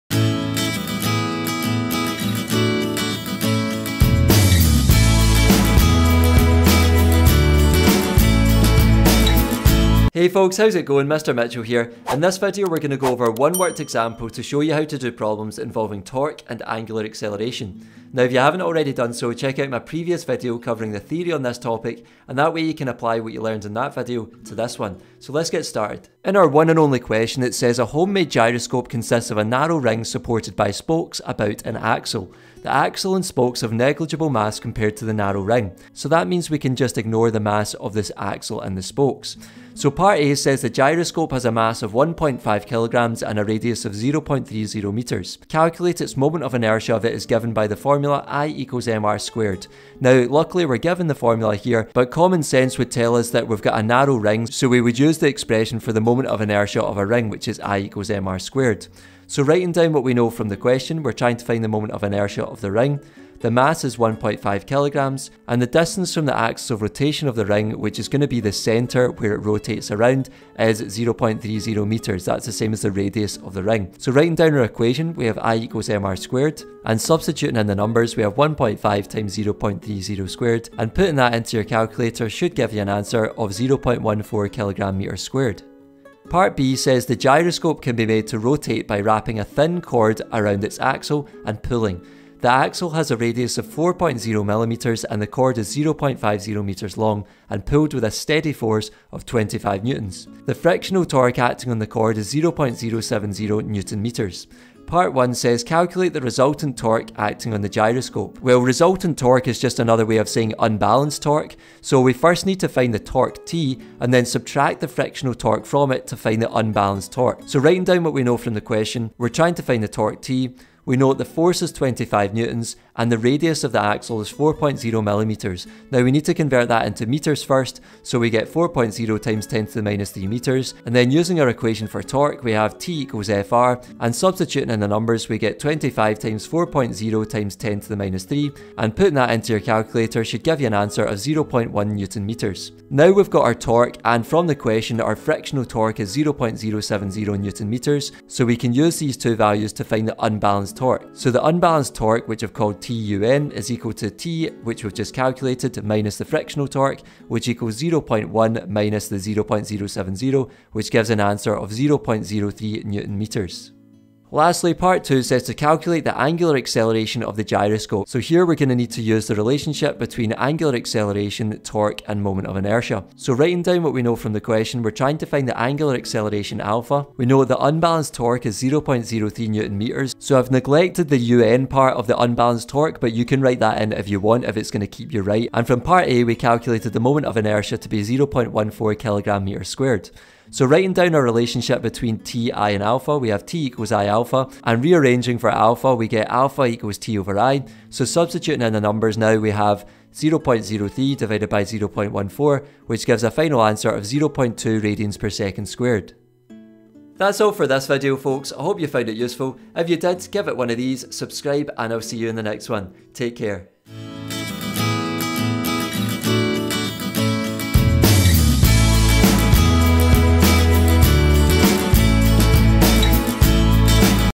Advanced Higher Physics | Rotational Motion | Torque & Angular Acceleration | WORKED EXAMPLES
In this video, I go over one worked example showing you how to tackle problems involving torque and angular acceleration.